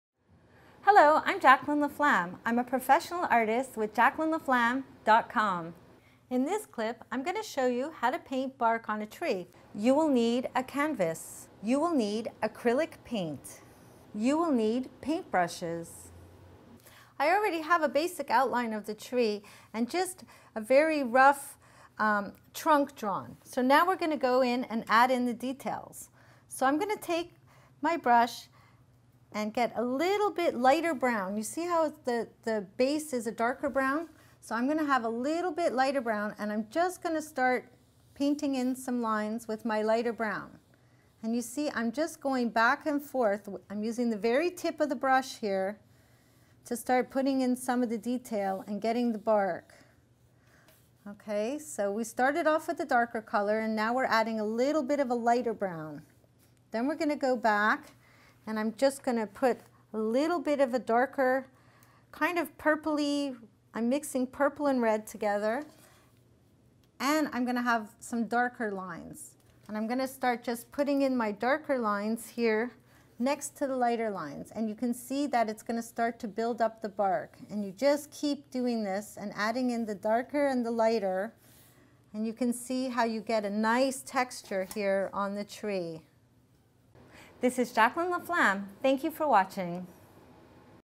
Acrylic Painting Tips : Acrylic Painting Techniques : Painting Bark on a Tree
To paint the bark on a tree, you should start with the darker base color and then add highlights over the top. Learn to paint bark on a tree with help from an award-winning artist in this free video on acrylic painting.

Bio: Jacklyn Laflamme is an award-winning artist and grant recipient, and is emerging in the public art scene with her vibrant murals.